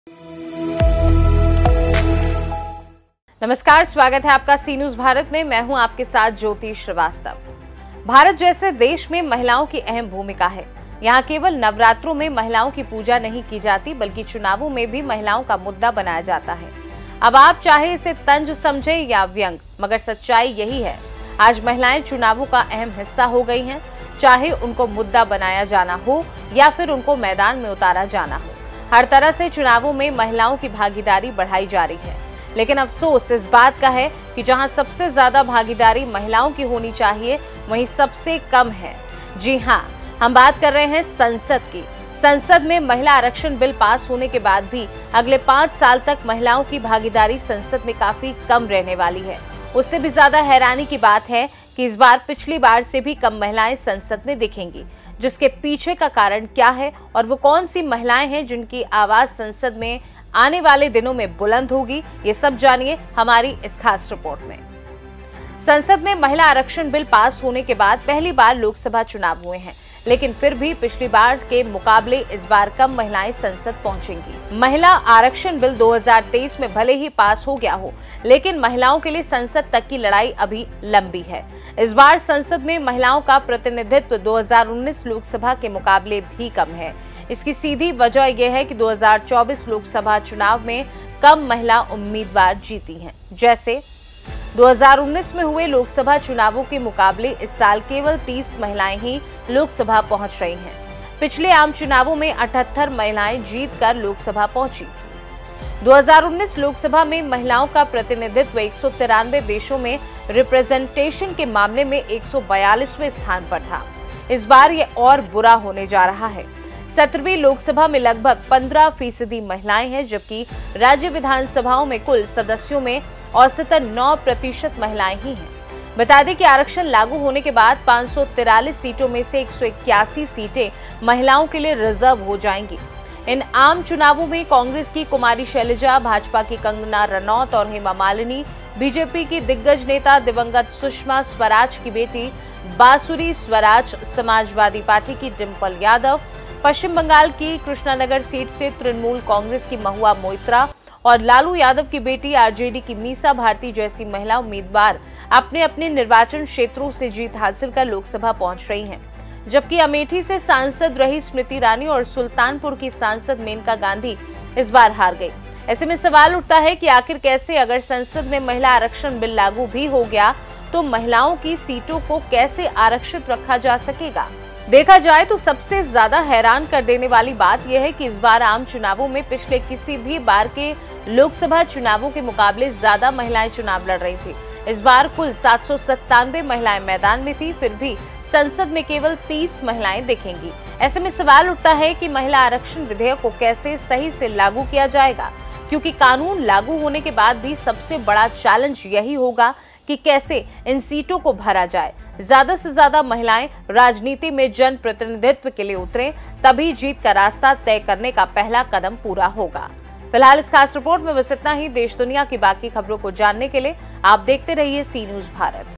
33% आरक्षण का कानून, फिर भी संसद में महिलाएं कम || Cnews Bharat
33% आरक्षण का कानून,  फिर भी संसद में महिलाएं कम  || Cnews Bhara